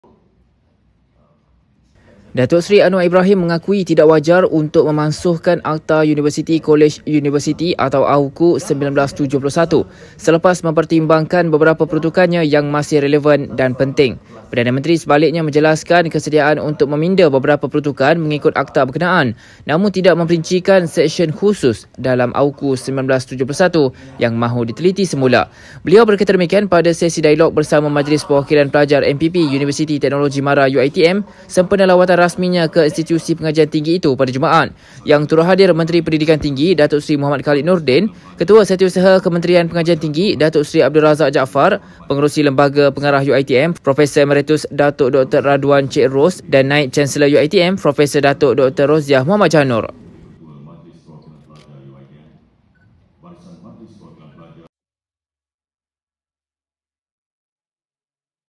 AUKU 1971 tak sesuai dibatal tetapi boleh dipinda ikut kesesuaian - Anwar
Datuk Seri Anwar Ibrahim mengakui tidak wajar untuk memansuhkan Akta Universiti Kolej Universiti (AUKU) 1971 selepas mempertimbangkan beberapa peruntukannya yang masih relevan dan penting.

Perdana Menteri sebaliknya menjelaskan kesediaan untuk meminda beberapa peruntukan mengikut akta berkenaan, namun tidak memperincikan seksyen khusus dalam AUKU 1971 yang mahu diteliti semula.